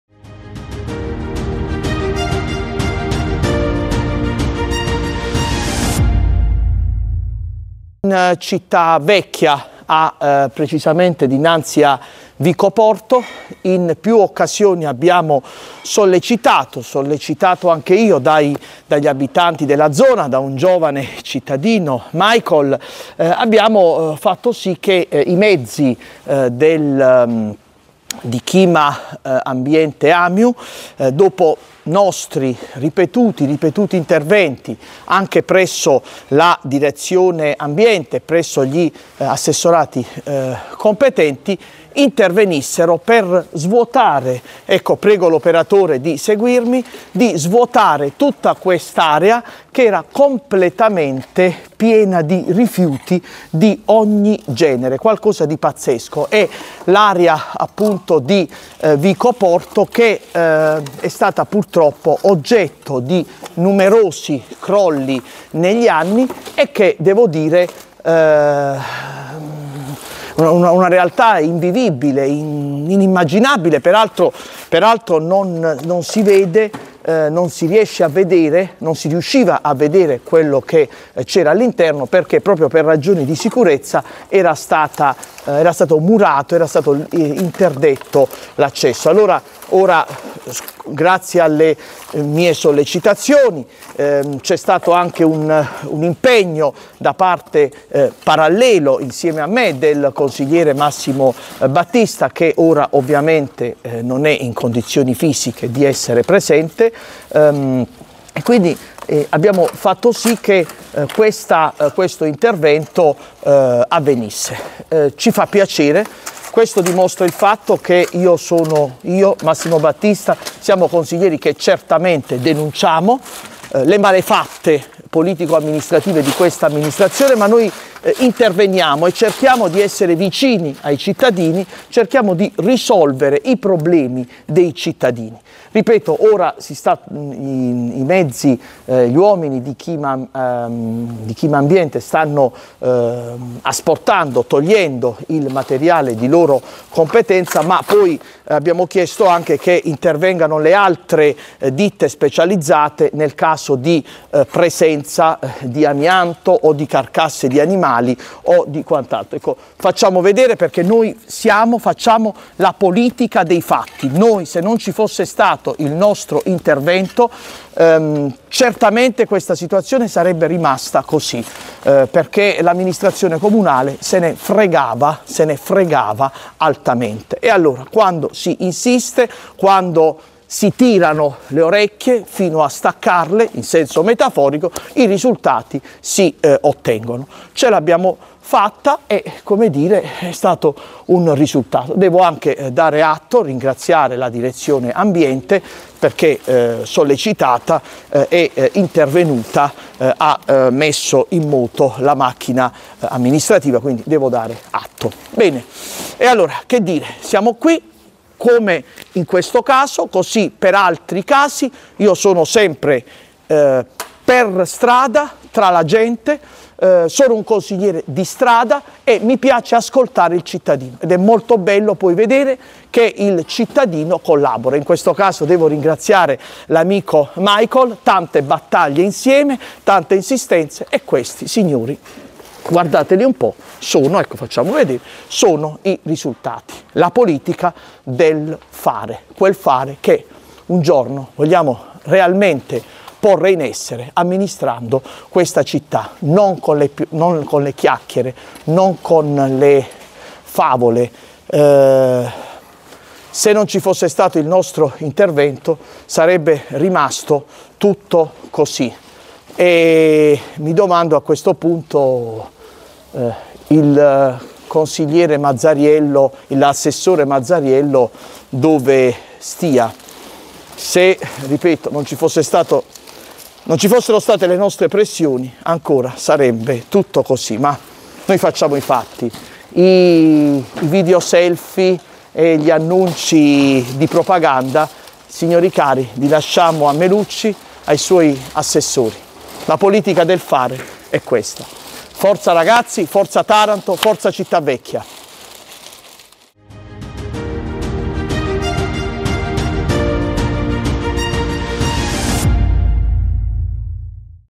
TARANTO VECCHIA - VICO PORTO: REALIZZATO INTERVENTO DI PULIZIA GRAZIE A L.ABBATE E M.BATTISTA
Taranto Vecchia - Vico Porto: realizzato un importante intervento di pulizia tra macerie e rifiuti di ogni genere.
L'impegno e le sollecitazioni di noi Consiglieri comunali Luigi Abbate e Massimo Battista hanno prodotto questi risultati: siamo "Consiglieri di strada" e la nostra è la politica del fare, non degli annunci e degli slogan tipici del sindaco Melucci & company.